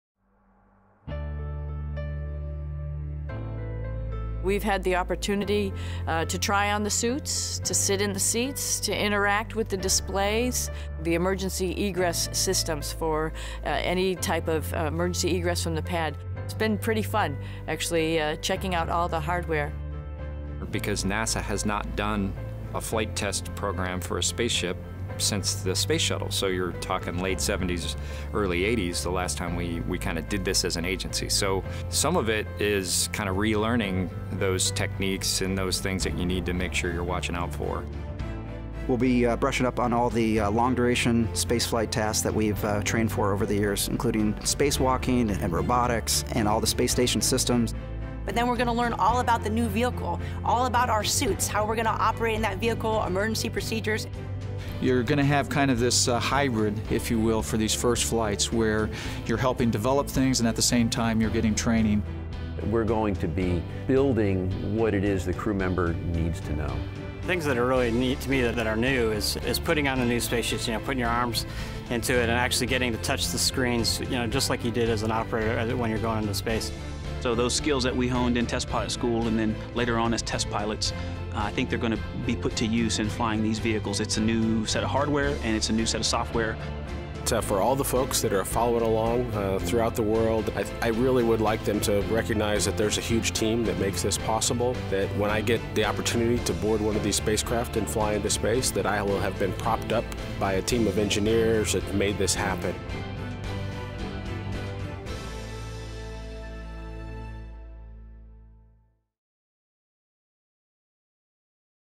Watch Commercial Crew Astronauts Train for Their First Space Flight
From trying on spacesuits to preparing for potential emergencies, see how astronauts are getting ready to fly on the test flights and first missions of Boeing’s Starliner and SpaceX’s Crew Dragon.